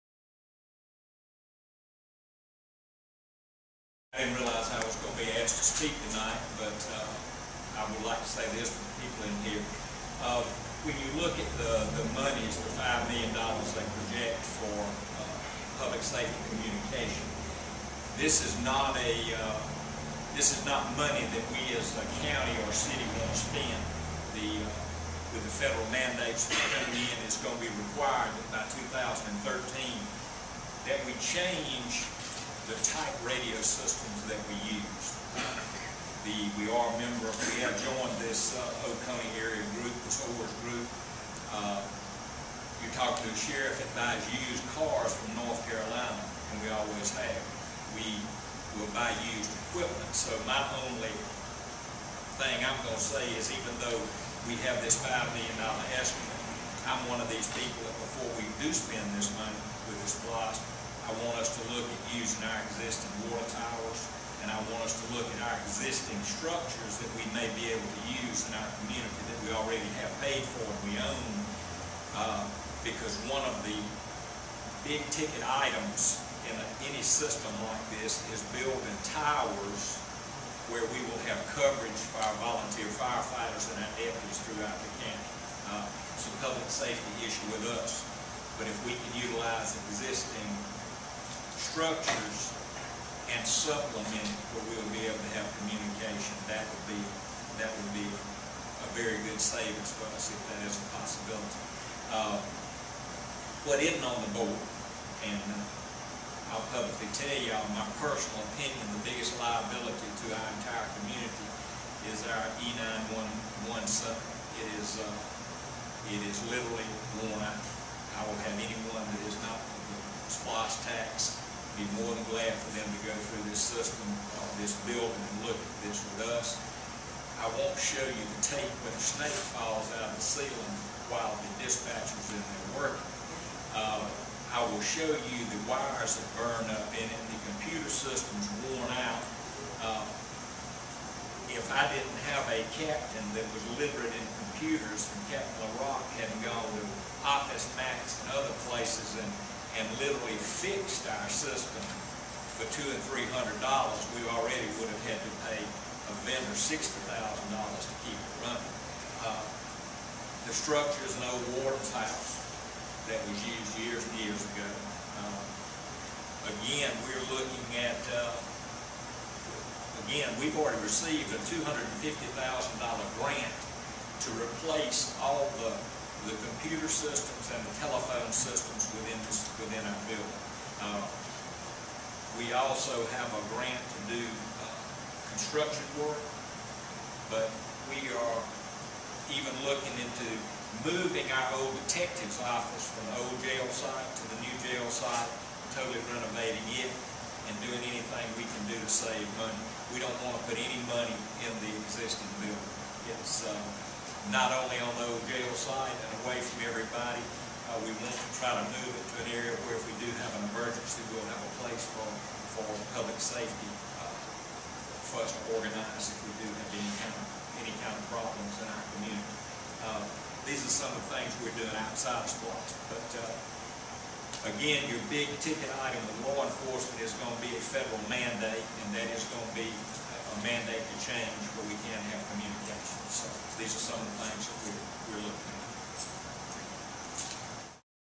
Bill Massee.m4v
Baldwin County Sheriff Bill Massee discusses the SPLOST revenue mechanism and its impact on the local law enforcement community.